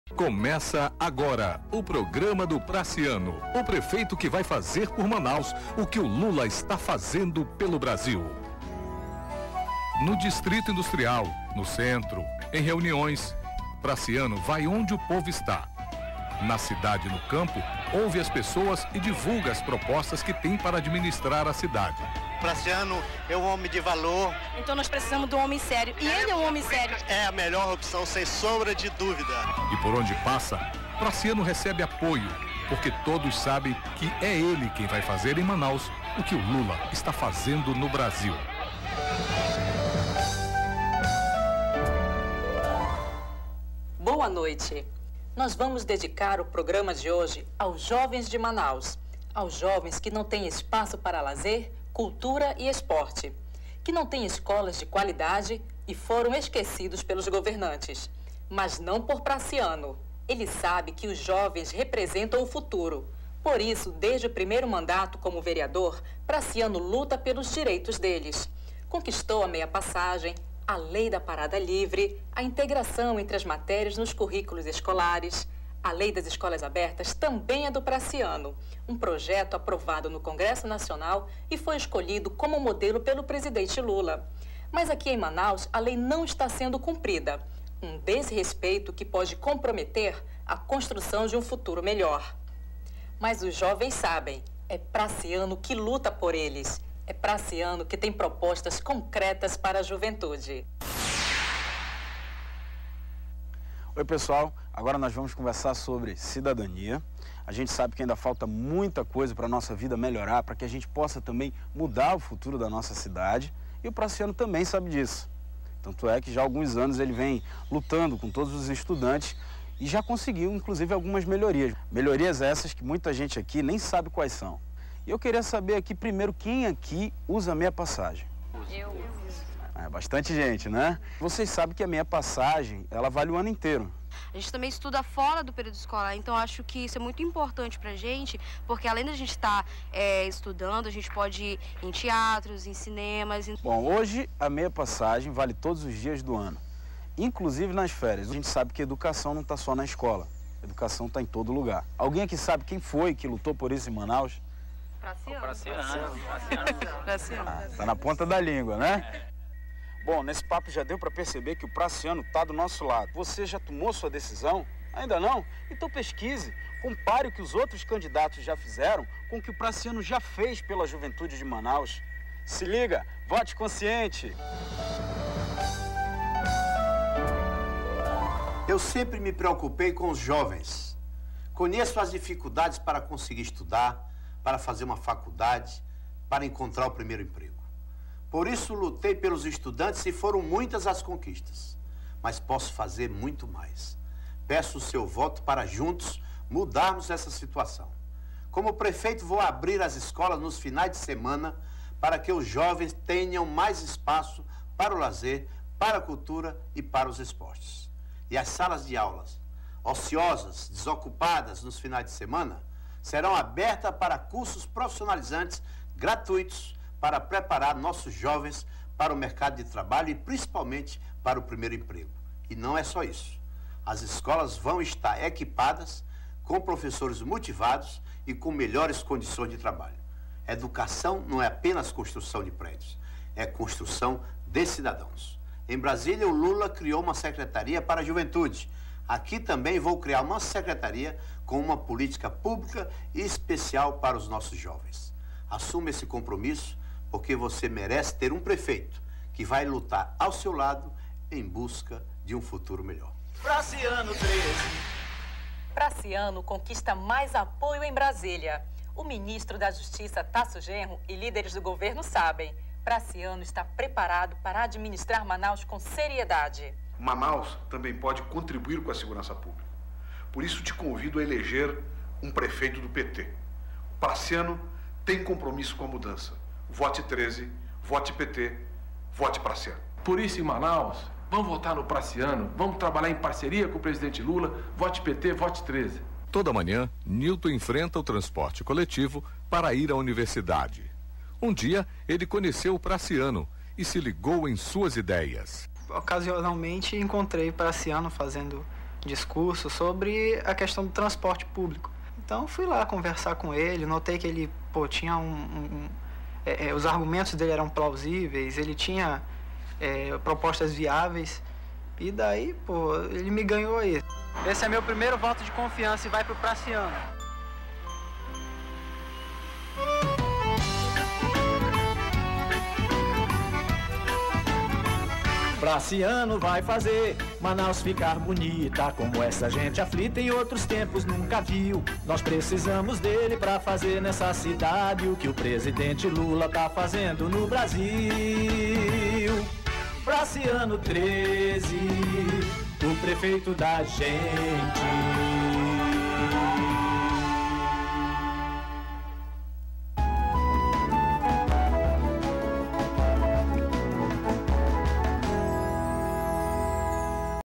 Francisco Praciano (PT) - Horário Eleitoral Gratuito
Candidato: Francisco Praciano
Vice: Luiz Castro
Coligação: A vitória do povo